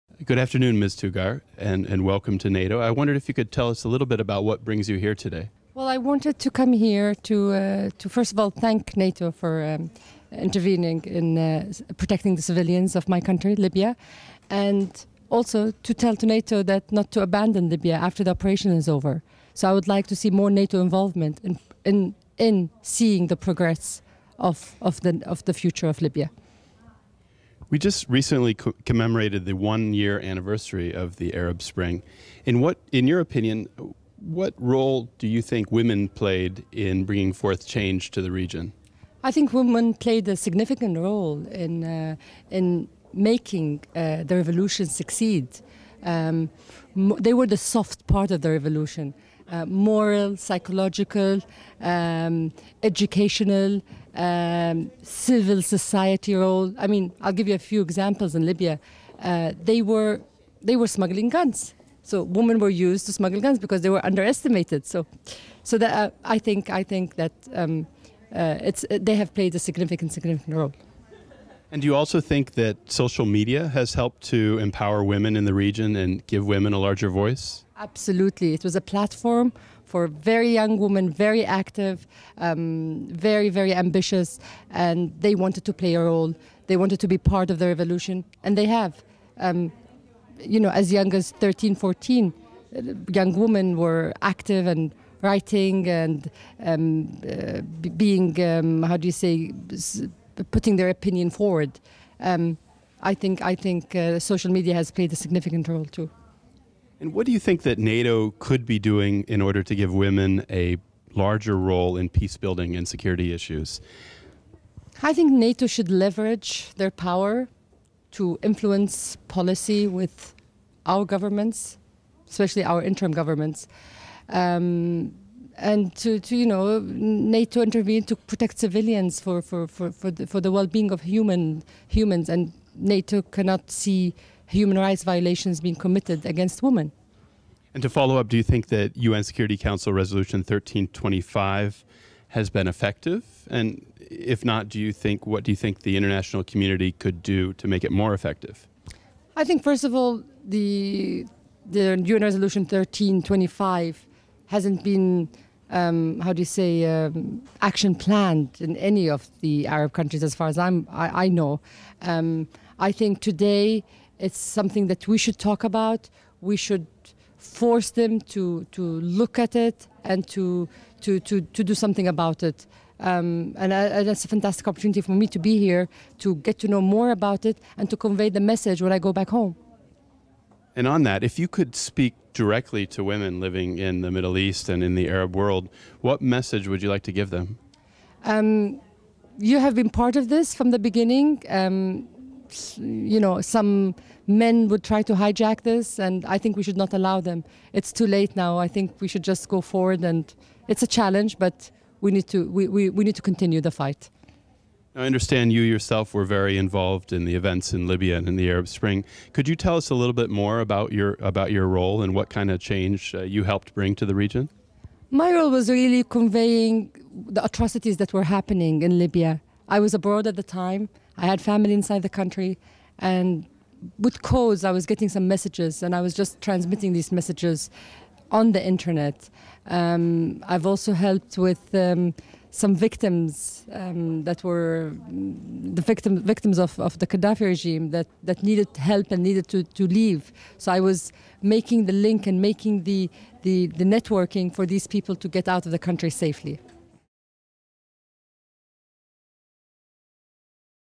WE-NATO: Ms. Maysson Amer Tughar (Human Rights Activist, Libya)
Ms. Maysson Amer Tughar asks NATO not to forget Libya, even though the mission had been terminated.

Ms. Tugar appeals for more involvement from NATO in the Libyan transitional government. She explains the pivotal role that women played in the Libya uprising from engaging in social media to smuggling guns under their clothes. When asked about United Nations Security Council Resolution 1325,. Ms.Tughar is optimistic  but recognizes that it has not yet been implemented into concrete action plans in the Arab states. She hopes to bring to Libya what she has learned from the conference..

Ms. Tughar also explains the role she played during the Libyan uprising, even though she was not in Libya at the time. Instead she helped her fellow countrymen by publishing messages sent from Libya in the internet and by aiding friends with obtaining visas. NATO Homepage